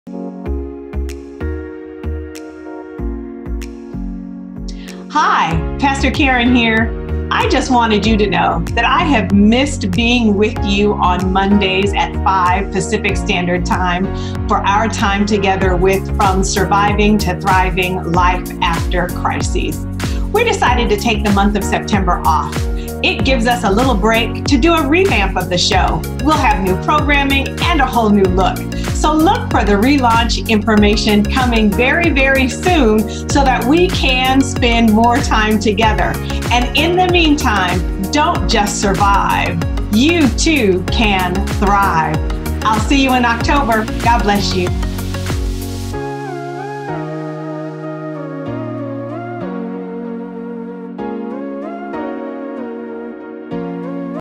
From Surviving to Thriving
An Update on the Surviving to Thriving Series.  If you have missed any episodes and would like to get caught up, find the entire series on the First Institutional Baptist Church YouTube channel!  See you in October!